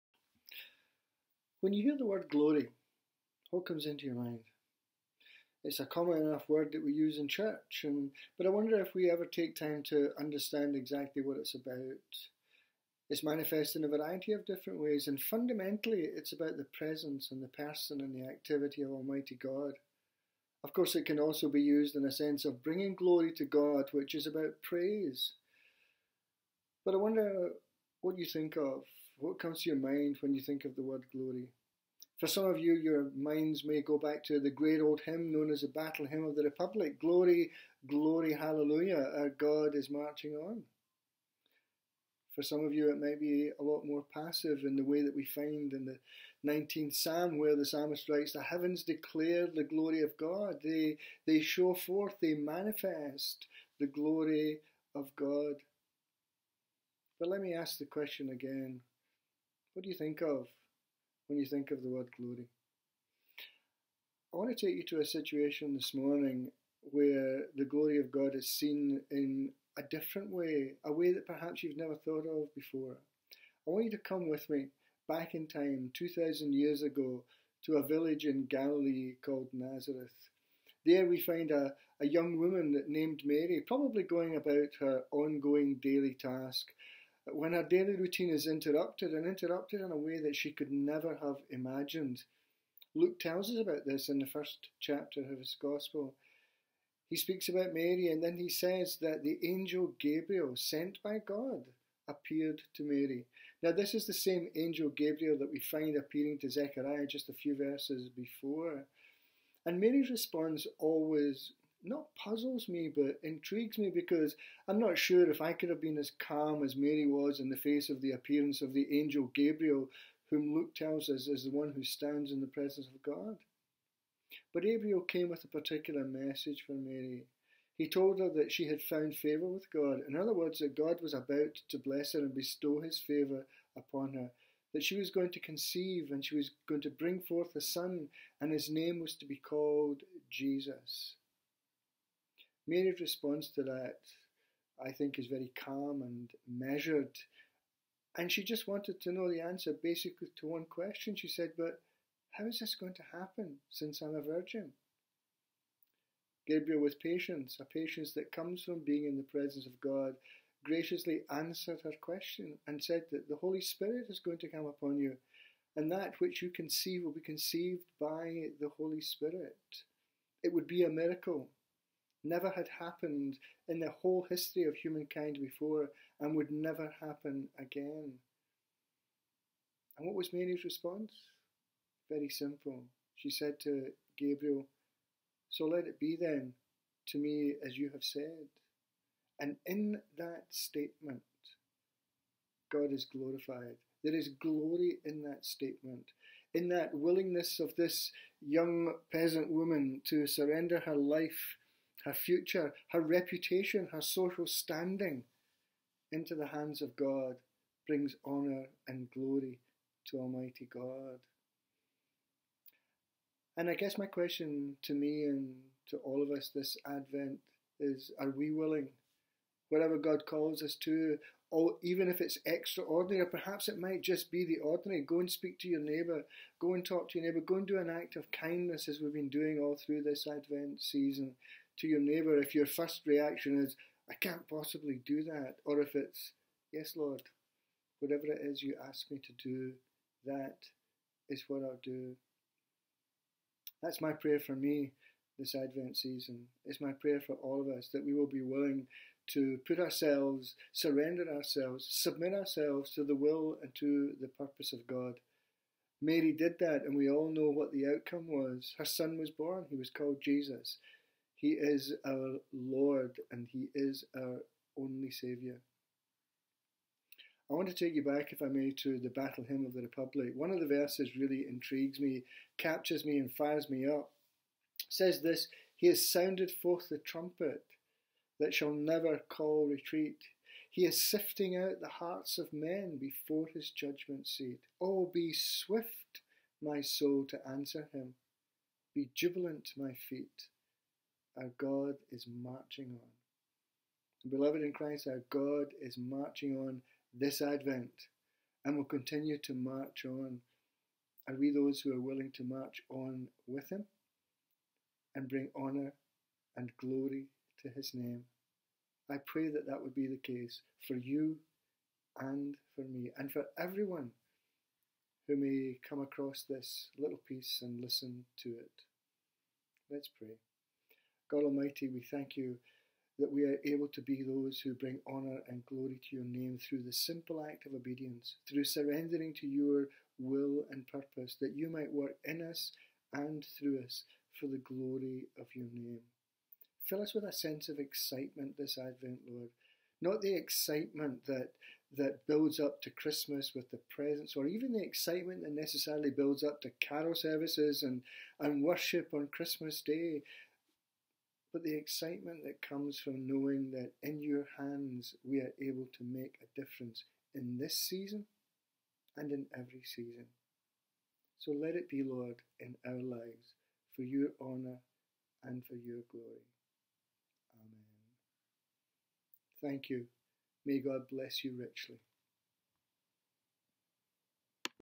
A 'Word' for Today - 5th December: 'Glory.'
Every day through Advent 2020 a team of 8 members of the Southside family will be reflecting on a single word that has been taken from the lectionary readings for Advent this year.  Please use these reflections to help you draw nearer to God as we move through this season of preparation at the end of a challenging year.